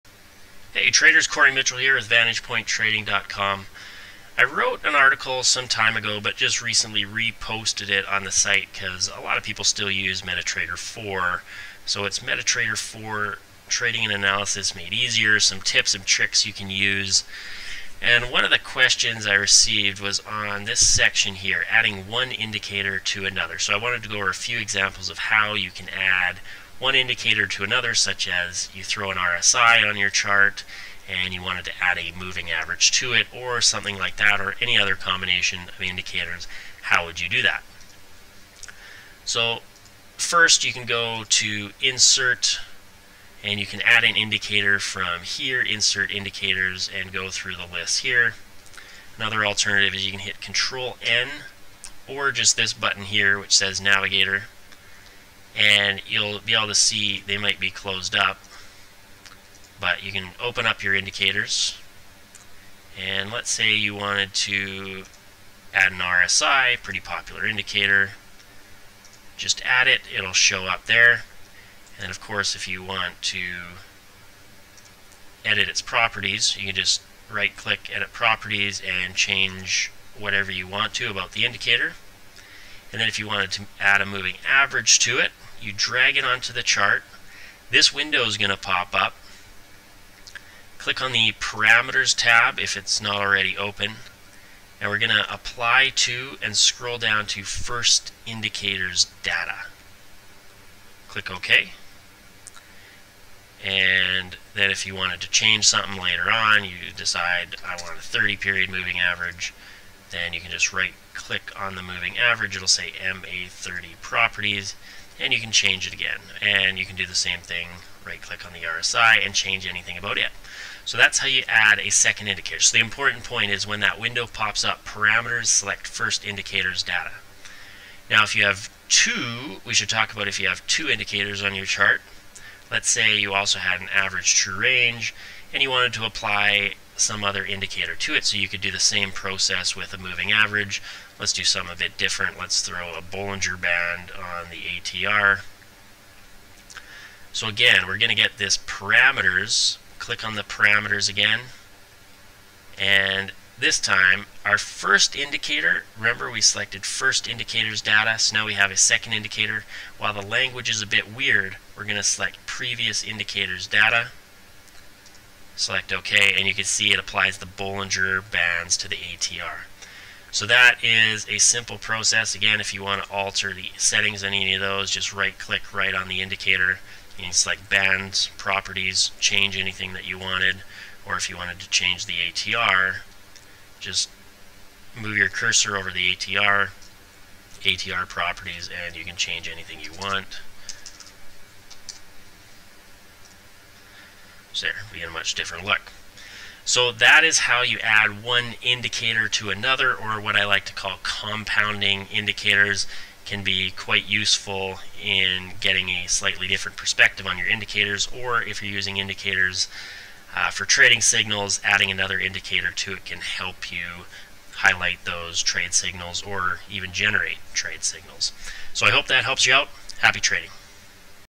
Adding One Indicator to Another Indicator in MT4
Here is how to  add one indicator to another in MT4. For example, if you have an RSI on your MetaTrader4 chart, and you want to add a moving average to the RSI, this video shows you how to do that.

The main steps are to:
-Add an indicator to your chart.
-Open your navigator window (or insert another indicator) and drag the indicator you want to add to the first indicator onto your chart. --A properties will pop up.  Click the Parameters tab.
-From the Apply To dropdown menu, select "First Indicator's Data" and that will apply the second indicator to the one already on your chart.